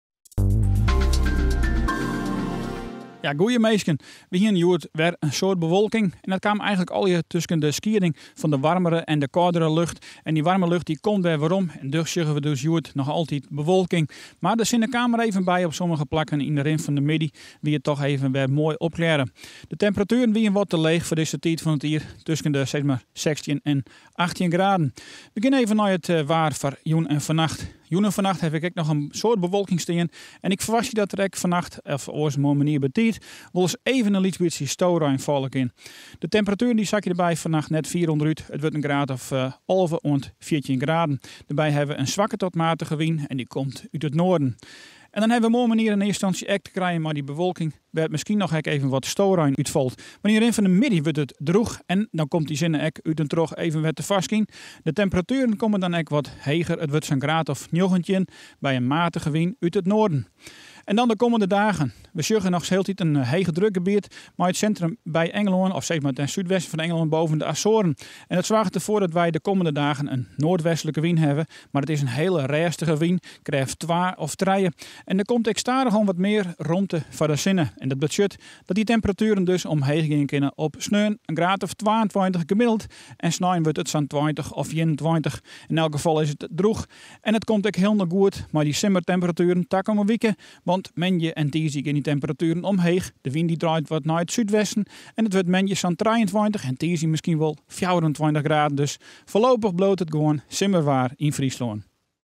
It waar fan 11 july 2013
Foarútsjoch
We starte mei bewolking en mooglyk earst út en troch wat storein. Fanmiddei wurdt it meast droech en komt de sinne stadichoan foar 't ljocht. It kwik rinnt dêrby op nei 19 of miskien pleatselik 20 graden, by in swakke oant matige noardelike wyn.

Sneon en snein geregeld de sinne en temperatueren tusken de 20 en 22 graden. Kommende wike wurdt it waarmer mei sinneskyn en droech waar op moandei en tiisdei. De temperatueren komme te lizzen om de 23-24 graden.